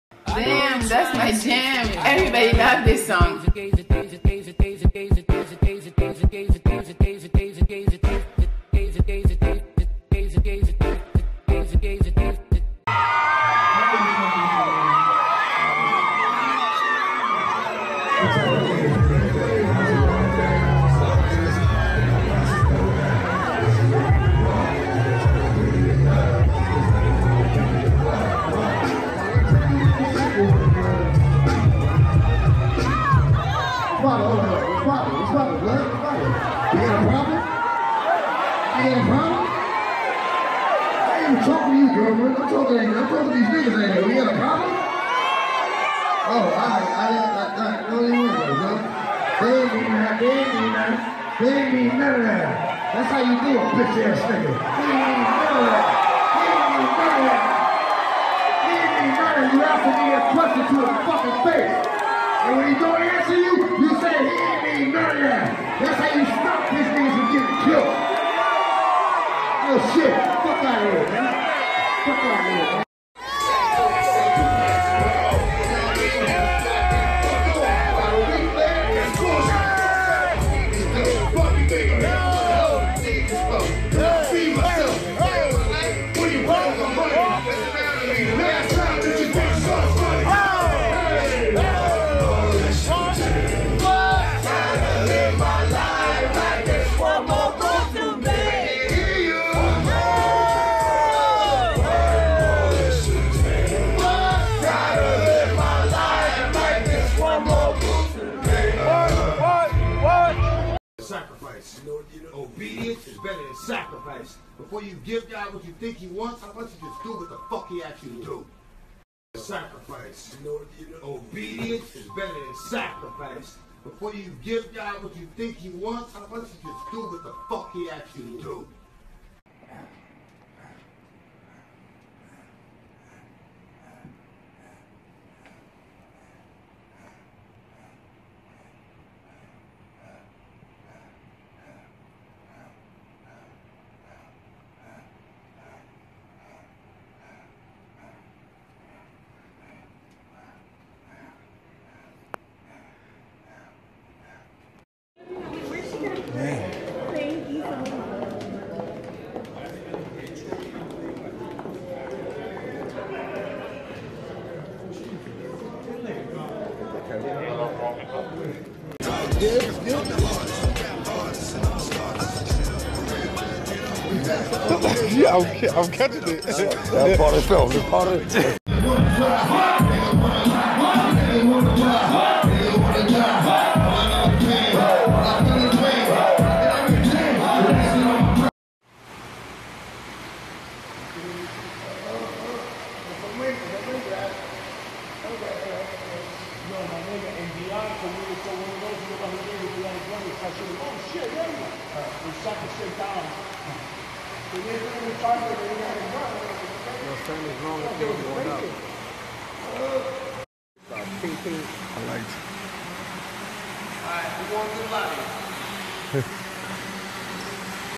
DMX Almost Fights A Fan After Being Disrespected On Stage At Concert
DMX Goes Of On A Fan After Being Disrespected On Stage At Concert